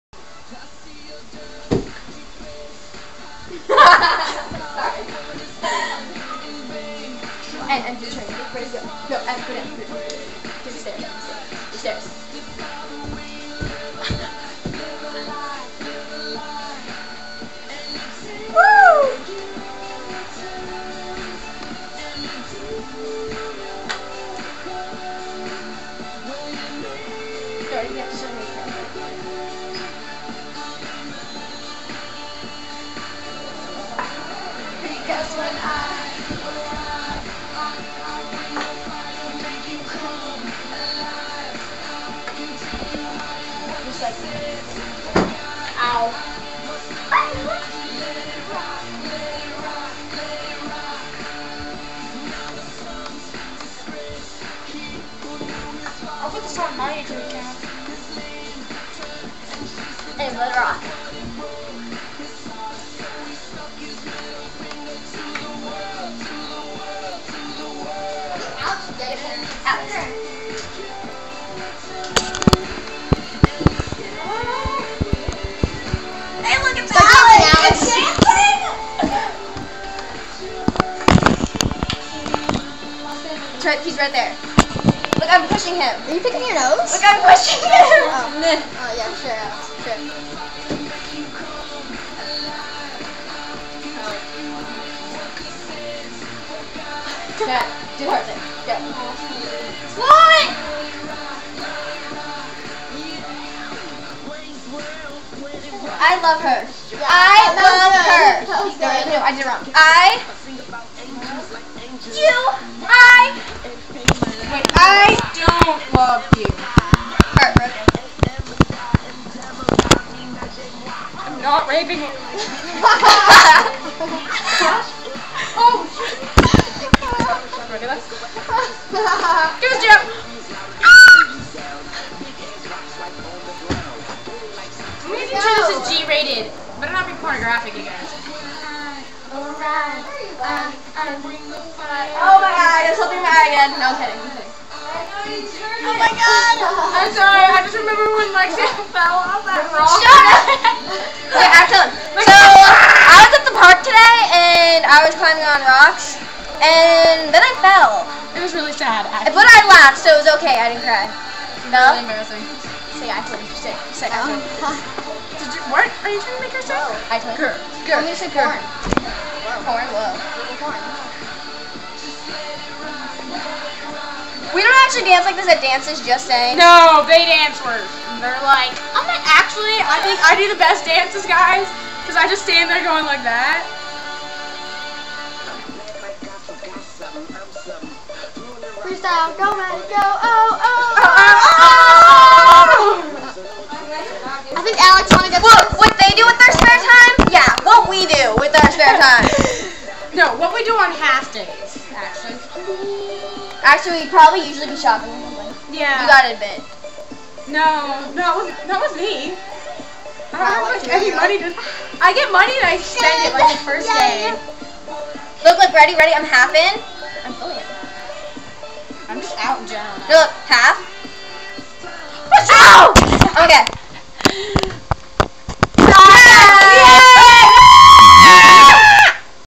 Emily and Lexie dance off
Emily and Lexie "challenge" Emmit and Mansa to a dance off...? Even though Emily and Lexie don't actually know Emmit and Mansa... We all know who will win